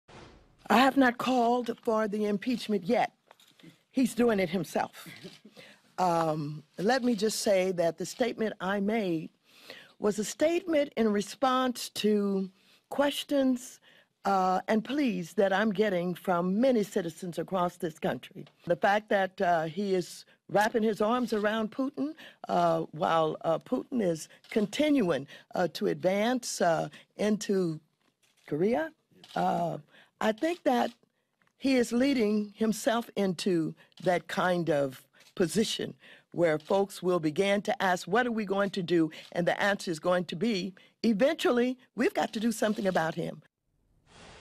Rep. Maxine Waters: Putin is continuing to advance into...KOREA!?
Rep. Brad Sherman's reaction (right side) is priceless.